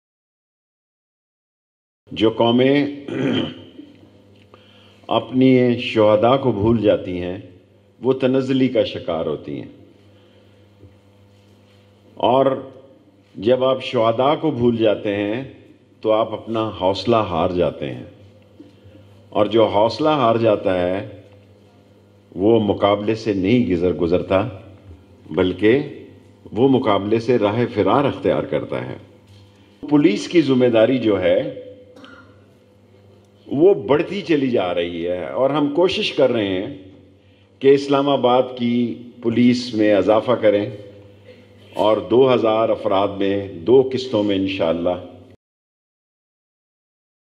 وفاقی وزیر داخلہ شیخ رشید احمد کا تقریب سے خطاب | Part 1
وفاقی وزیر داخلہ شیخ رشید احمد کا تقریب سے خطاب

اسلام آباد کی پولیس فورس میں اضافہ کرنے جا رہے ہیں